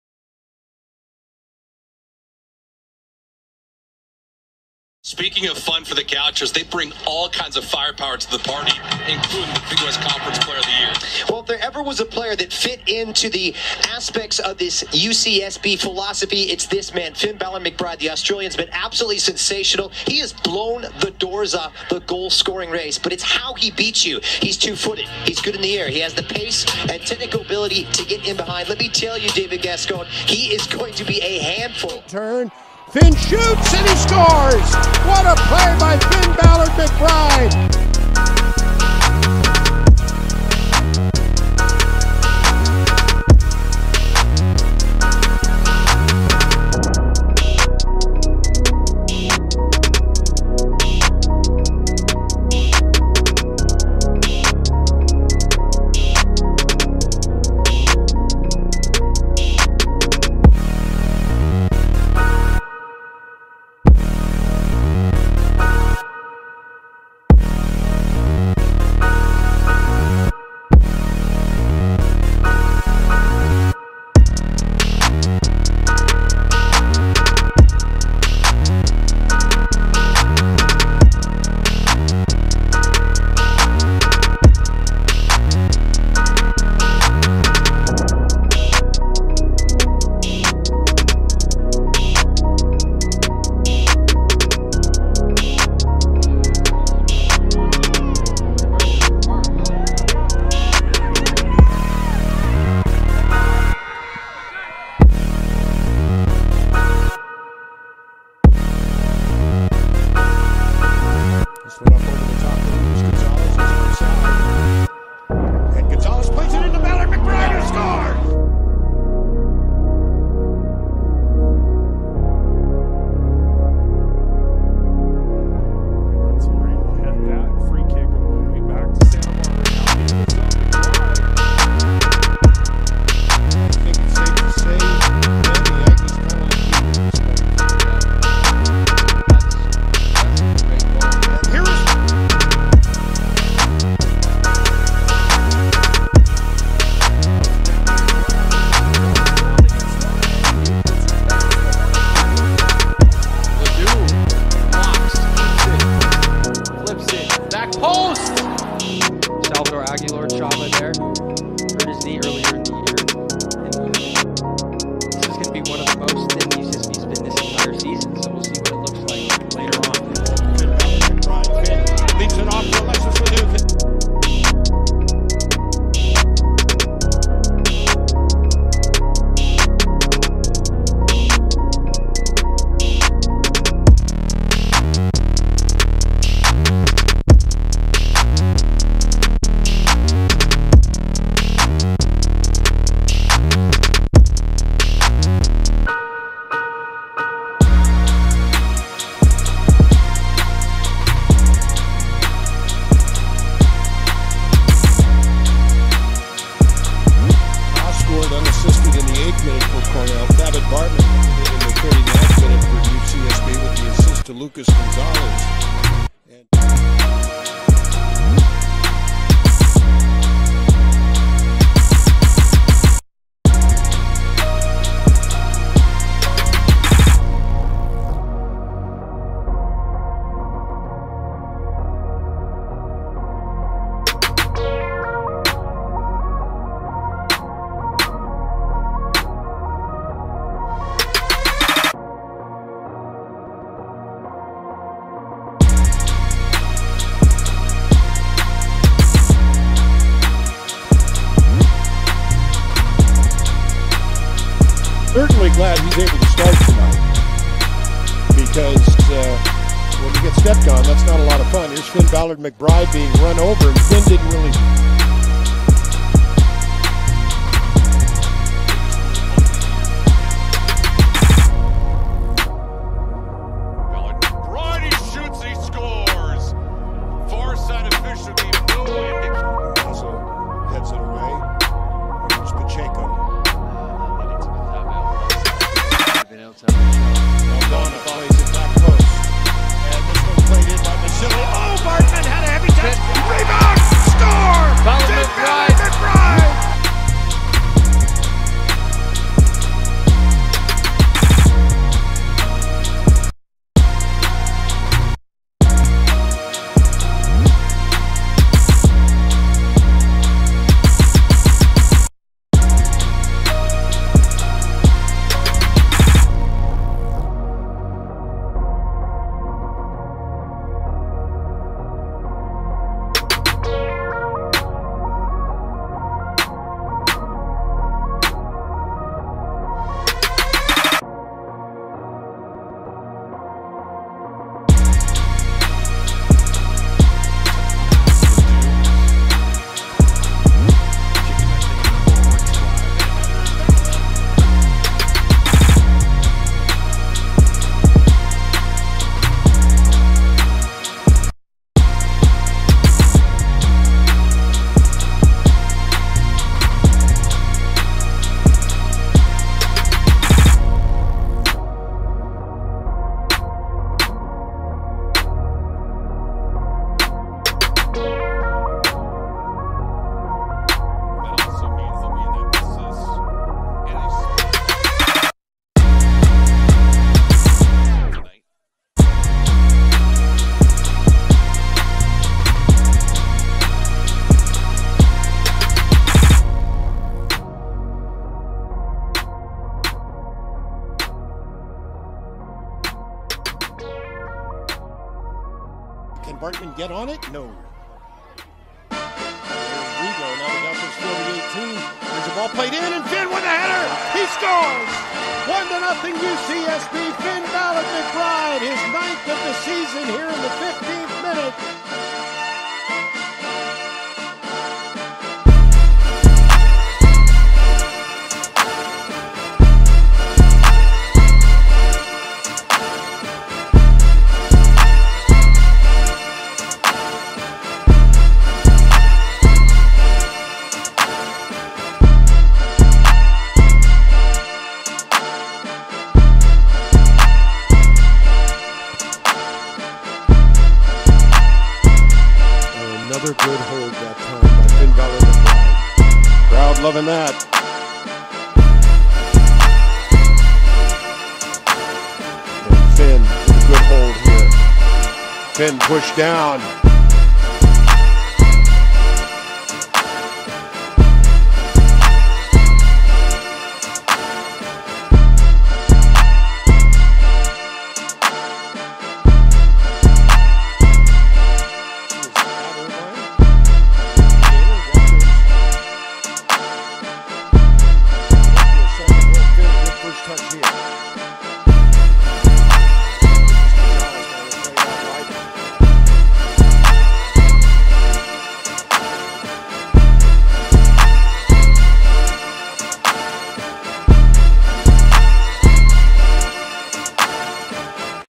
Finn Ballard McBride UCSB College Highlights
Top Goalscorer in NCAA D1 over the past 3 Seasons
Graduating Senior
MAC Hermann Trophy Watchlist
All American 3rd team
All-Far West Region 1st Team
Big West Offensive player of the year
UK Ancestry Visa